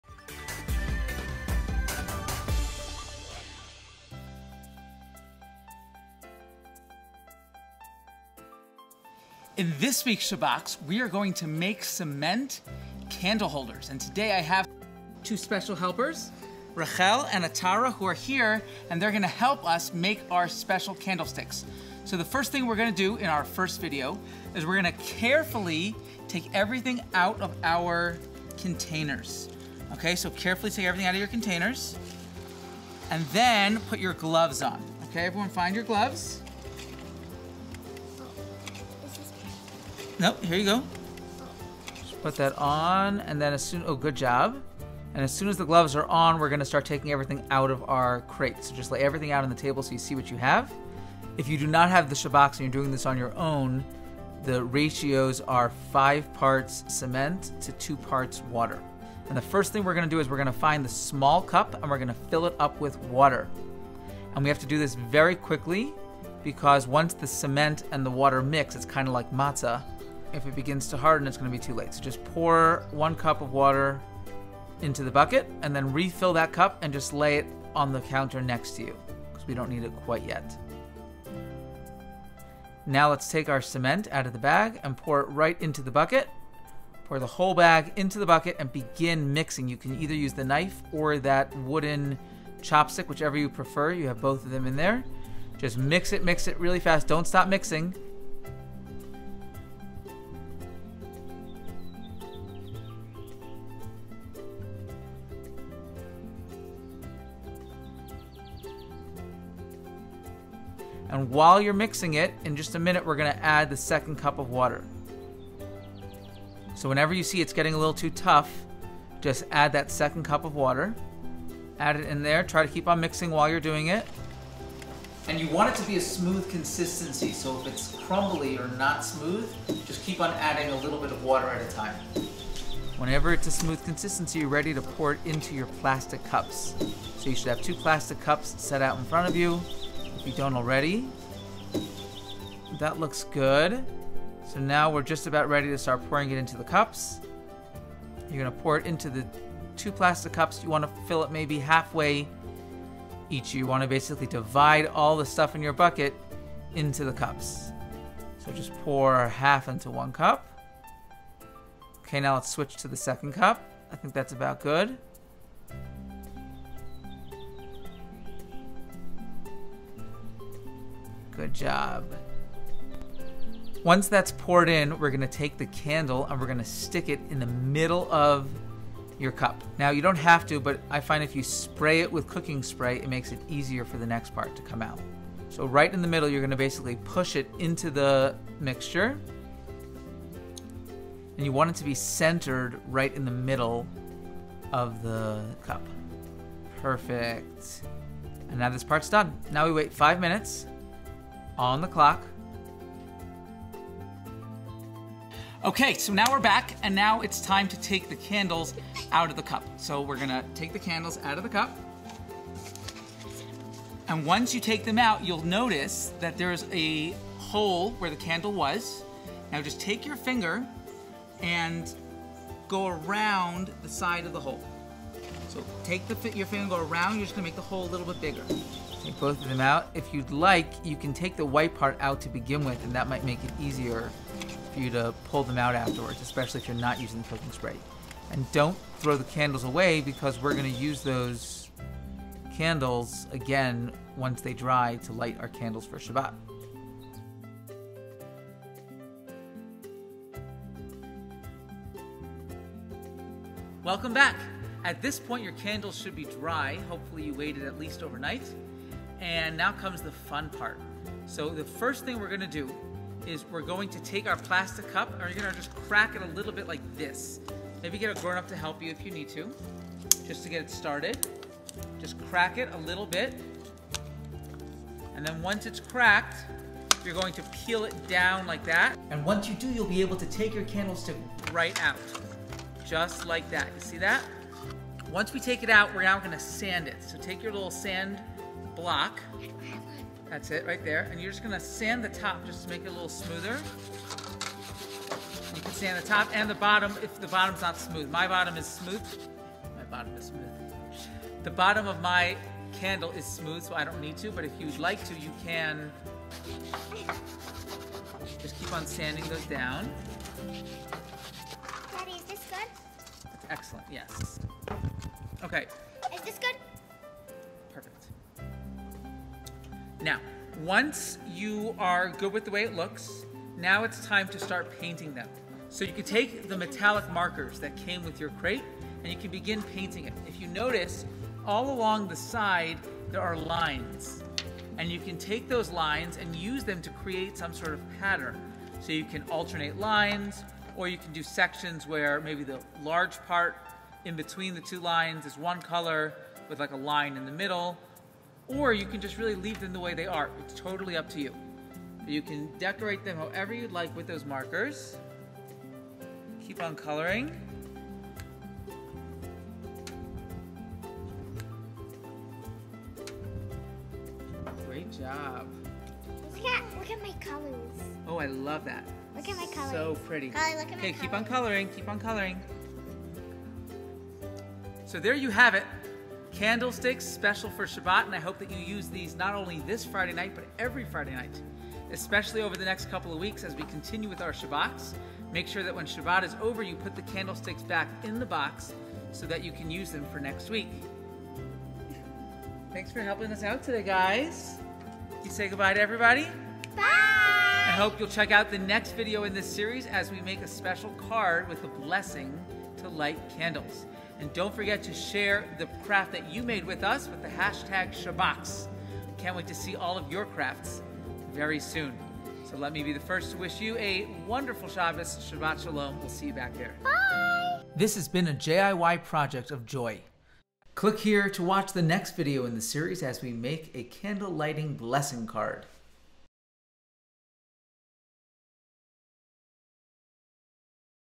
Cement Candle Holders
In our second Shabbox Crate we make cement candlesticks. Visit jiy.family for more resources if you are doing this craft without the Shabbox.